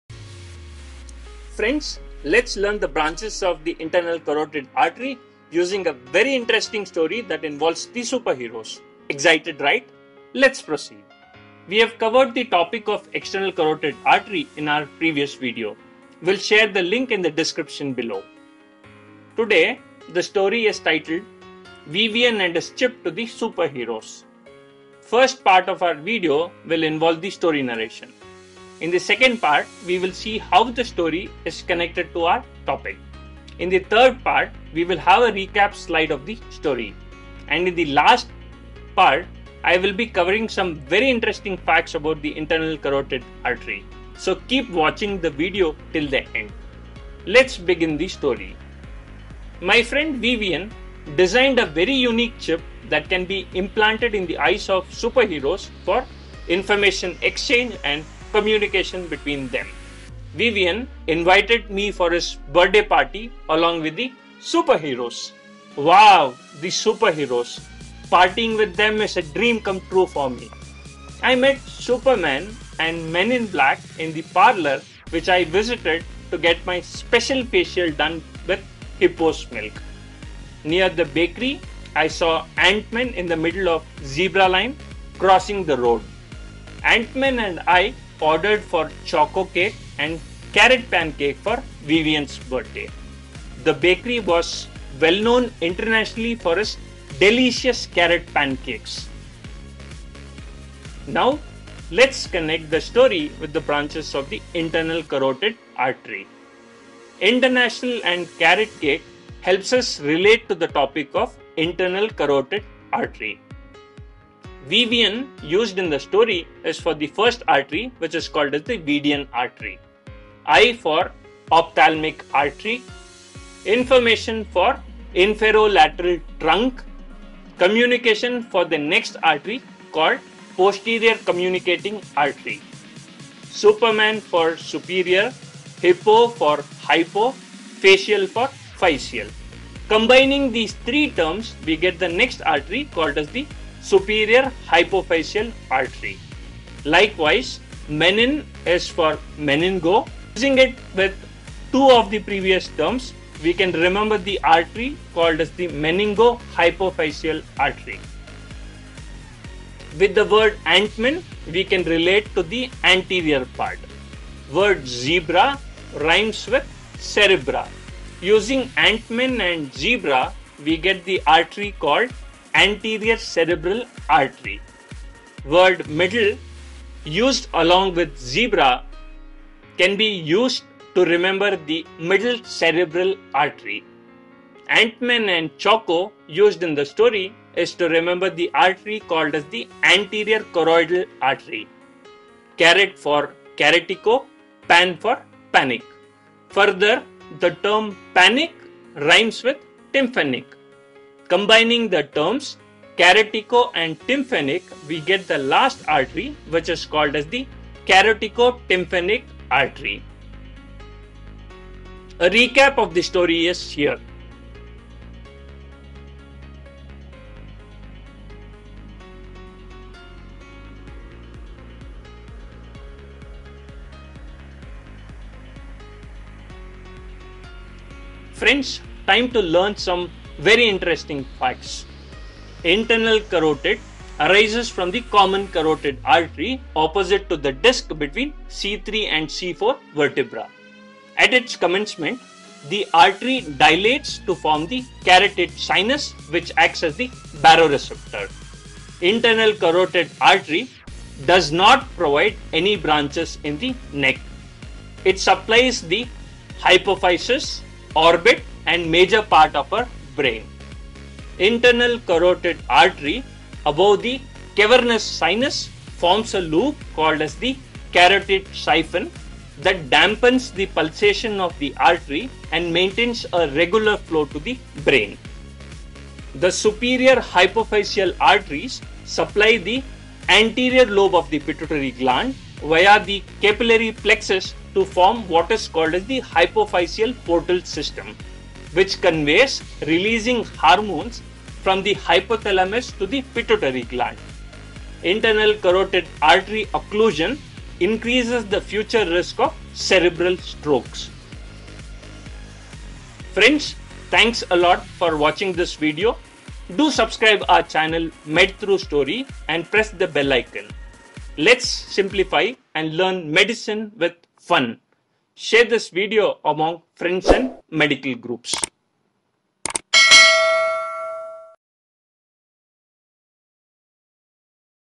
Easiest trick for Internal Carotid Artery in just 3 mins | MedThruStory
External Carotid Artery branches in just three minutes.

The most unique and creative way of remembering Internal Carotid Artery branches in just 2 minutes.

MedThruStory promotes easy and fun learning for medicos using stories.